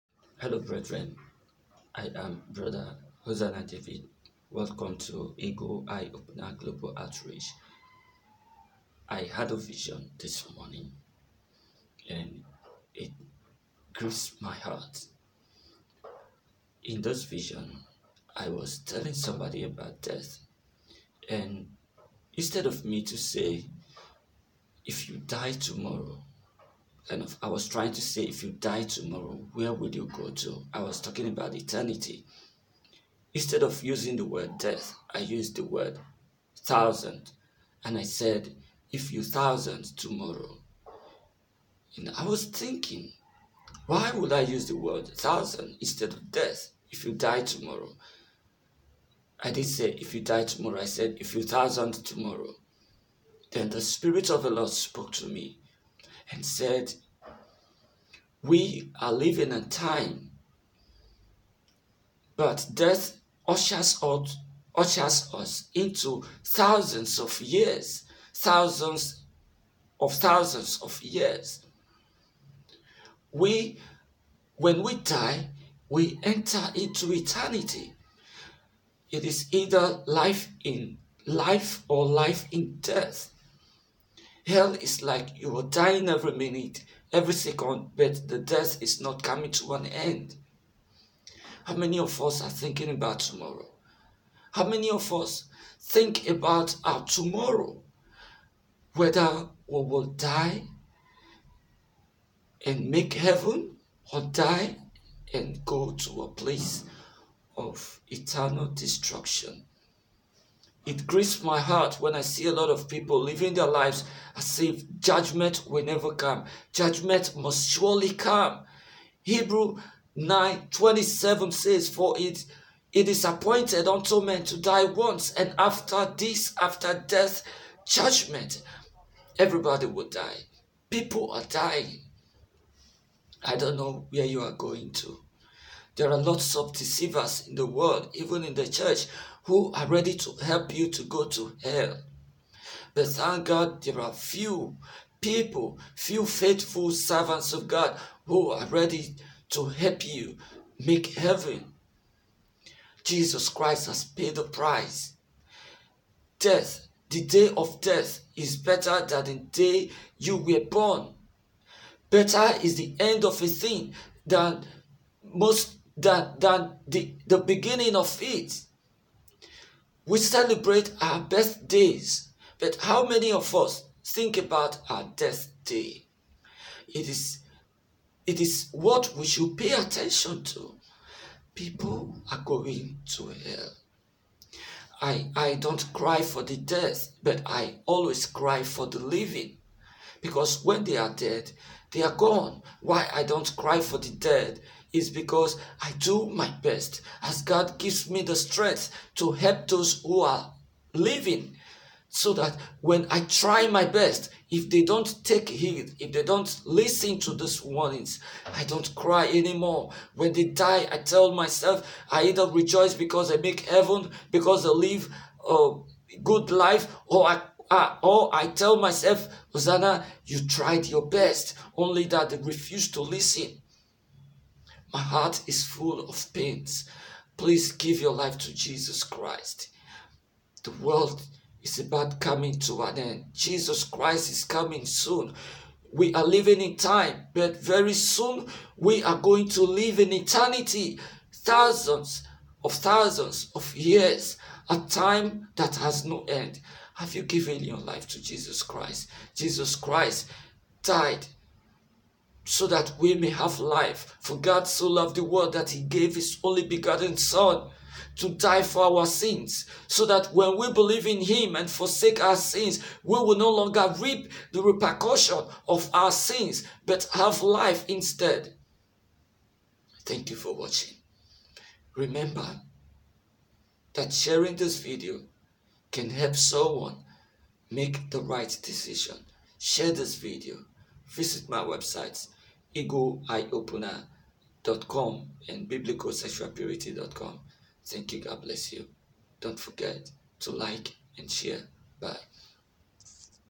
"When You Thousand Tomorrow": an End Time Vision
In a vision I was talking to someone about death and the need to prepare. Instead of me to say “if you die tomorrow” I said “if you thousand tomorrow”. As I was wondering why I said “if you thousand tomorrow” the Spirit of the LORD spoke to me that the other side of life where death ushers us into is not a matter of a few years but of thousands of thousands of years. Death takes us into eternity, but there are just two destinations: eternal life and eternal death. Are you ready?